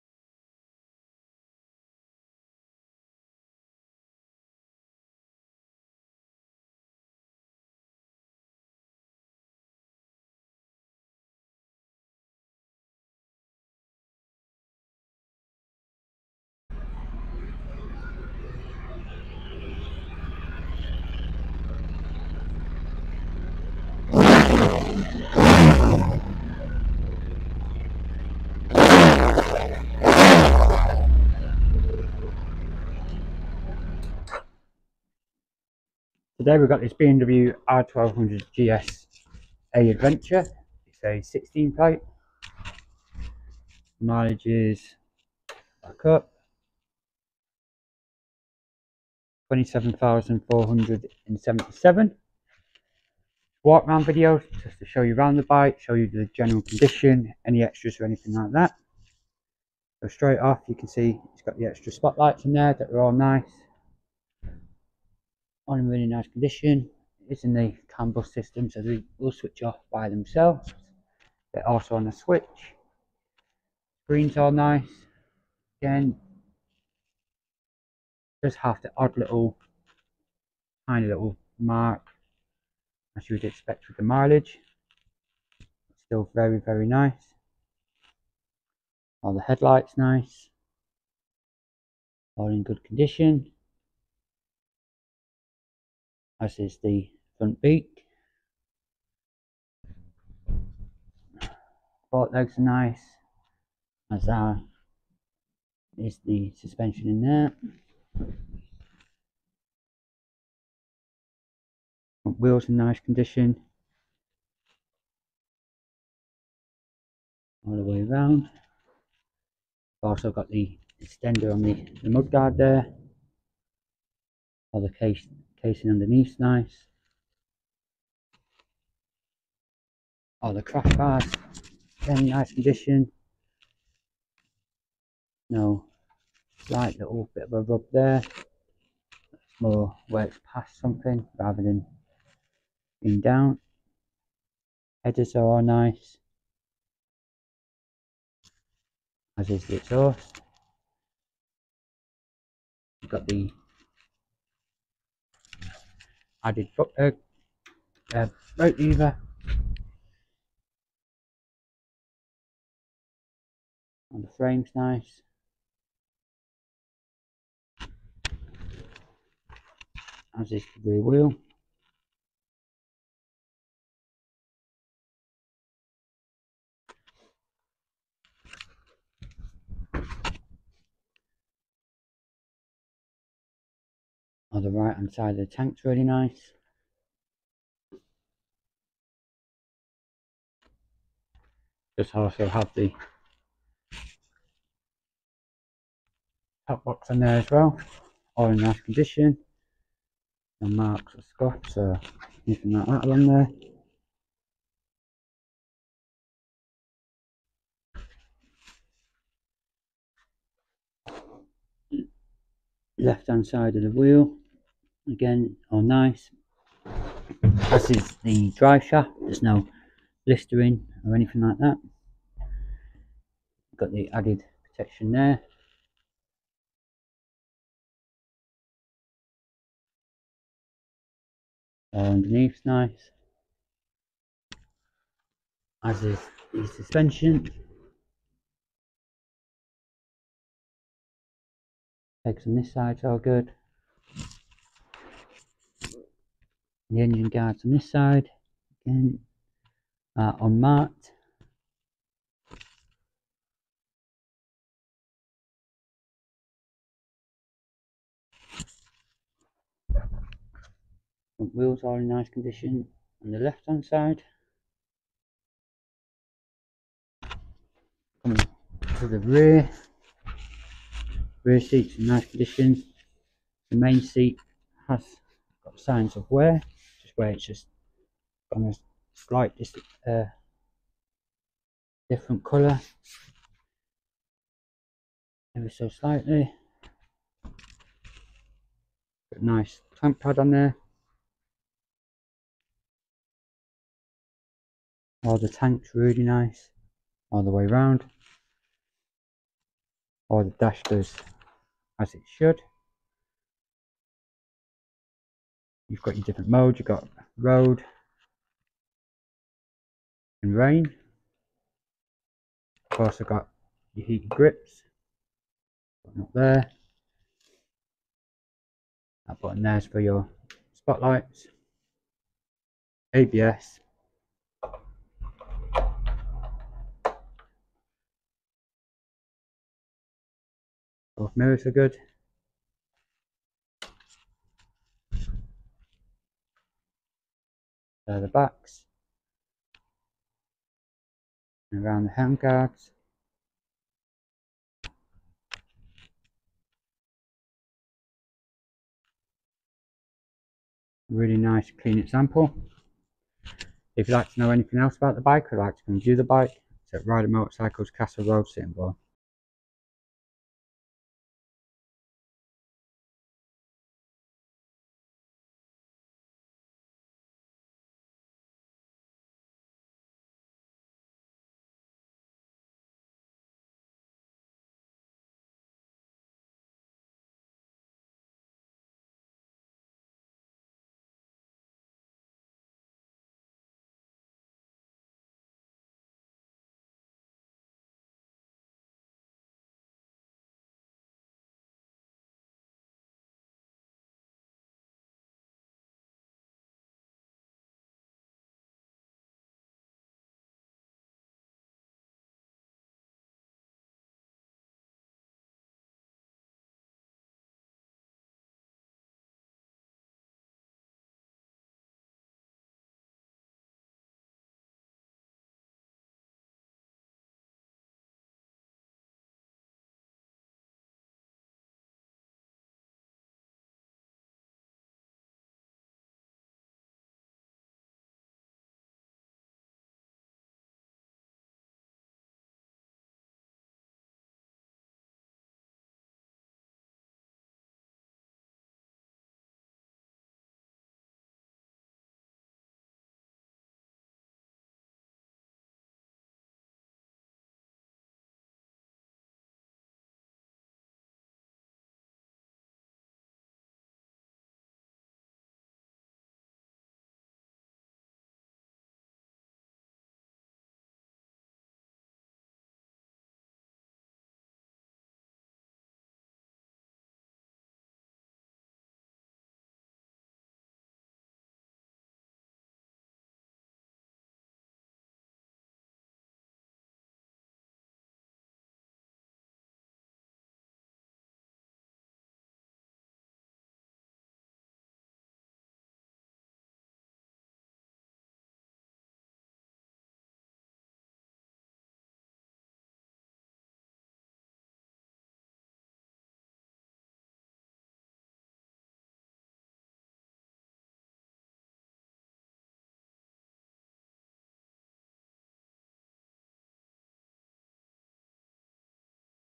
2016 BMW R1200GS Adventure - Used Bike Video Tour
We have this 2016 BMW R1200GS Adventure in stock now. Come and view it for yourself in store or buy